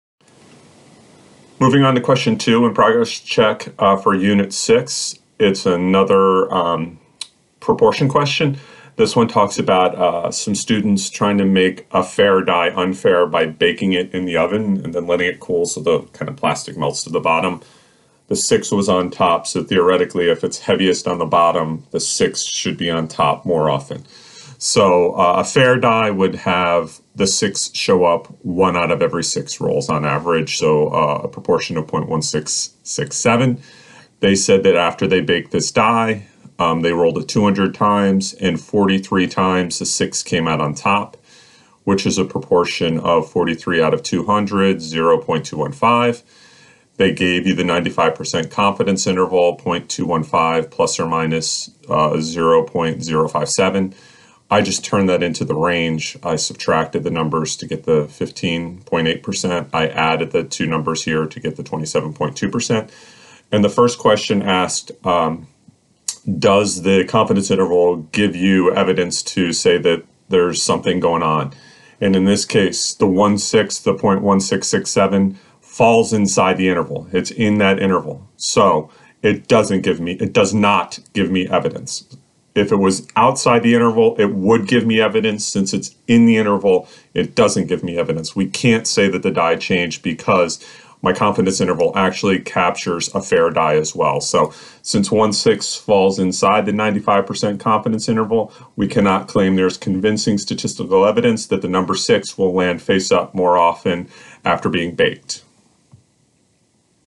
AP Statistics Unit 6 Progress Check 2(a)
Mr. Oliver discusses one-sample t-interval for true mean and how to make significance decisions.